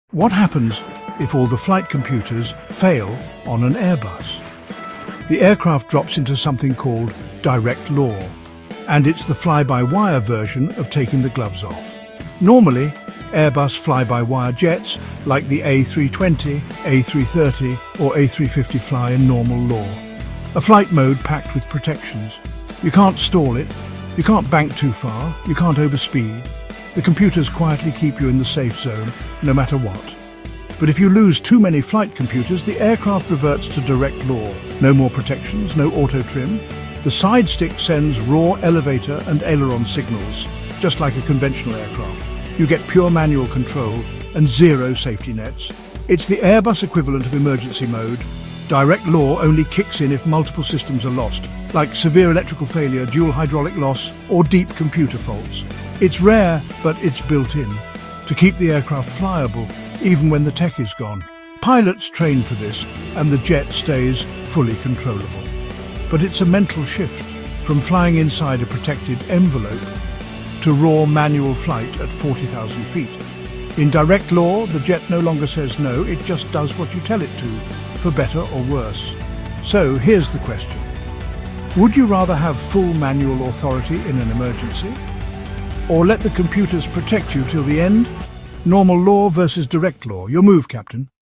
Airbus Direct Law: When Pilots Take Full Control at 40,000 Feet
Airbus “Direct Law” — automation off, raw pilot control on.
When the flight computers fail, protections vanish, and the jet flies exactly as commanded. Pure authority, no safety net.

Would you rather trust automation or go full manual at 40,000 feet?